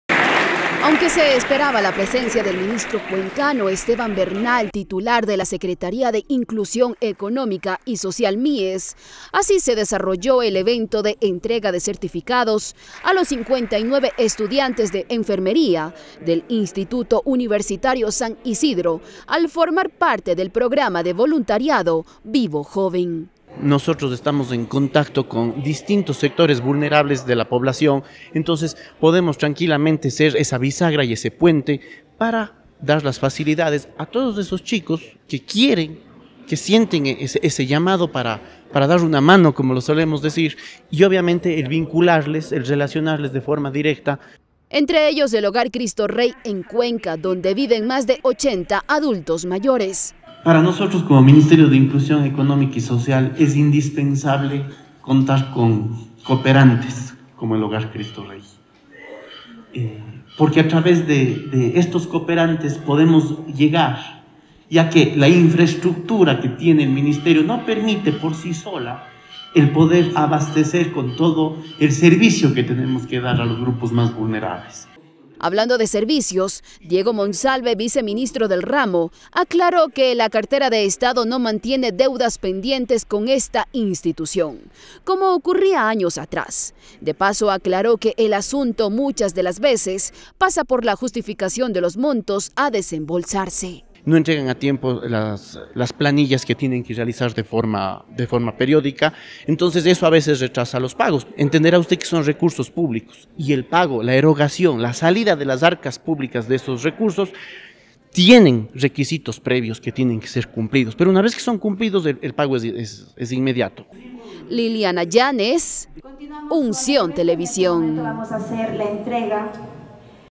Programa “Vivo joven” llega a más sectores de la población
A través del programa “Vivo joven”, el Ministerio de Inclusión Económica y Social busca llegar a más sectores de la población. En Cuenca, en el Hogar Cristo Rey que brinda ayuda a adultos mayores, un grupo de 59 estudiantes de Enfermería culminó sus prácticas pre profesionales. Con esto, el Estado aspira beneficiar a más grupos sociales, con el respaldo de la empresa privada.